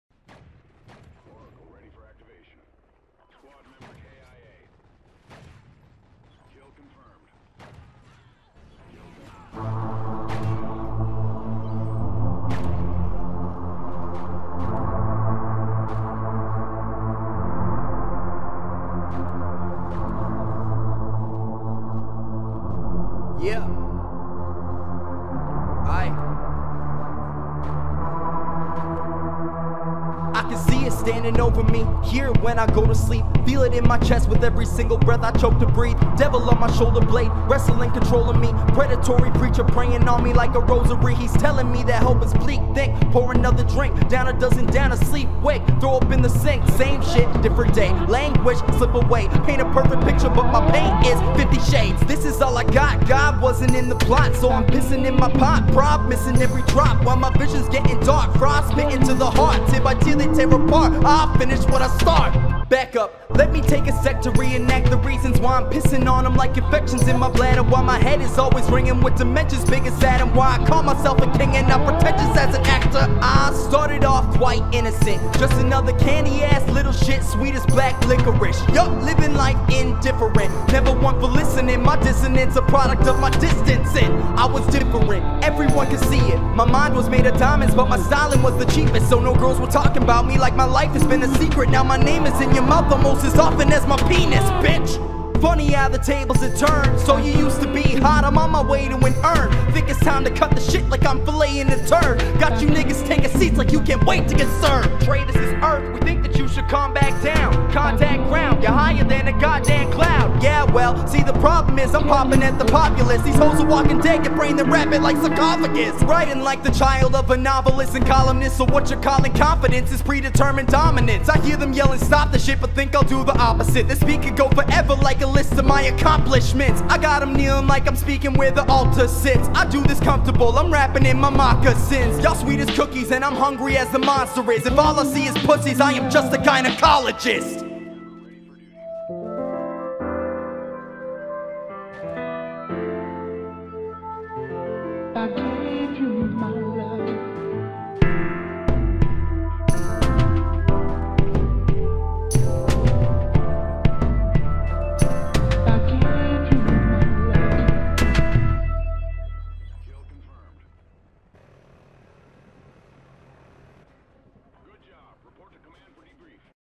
PAMAJ (A2) Montage Challenge Response @pamaj (54 kill sniping Gametage)
Only had two days and was reg gunning a lot too but wanted to show support for Pamaj and take part so although its not a traditional montage its a "game"tage using the A2 clan tags.  The 5 man at the end starting with a collat was pretty rewarding ;-)